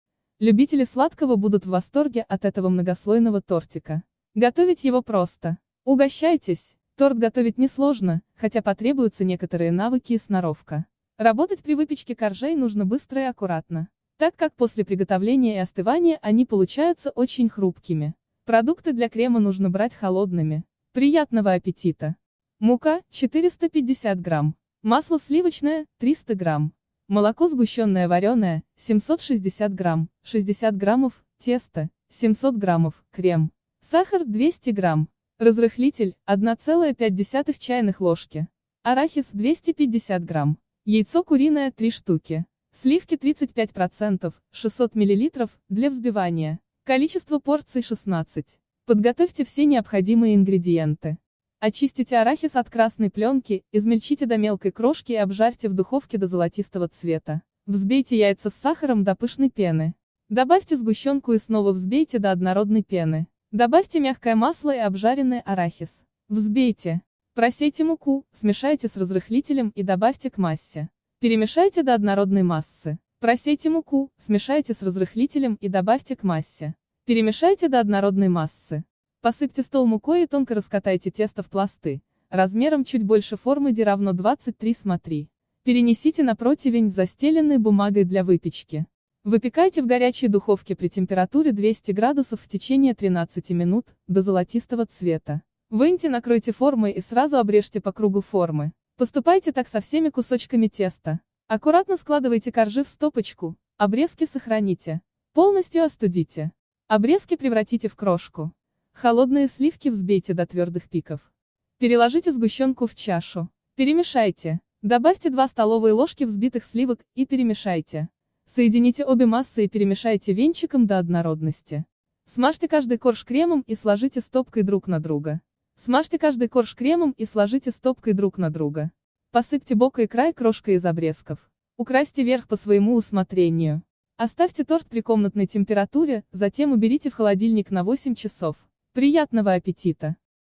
Арахисовый торт Коровка
Любители сладкого будут в восторге от этого многослойного тортика. Готовить его просто. Угощайтесь!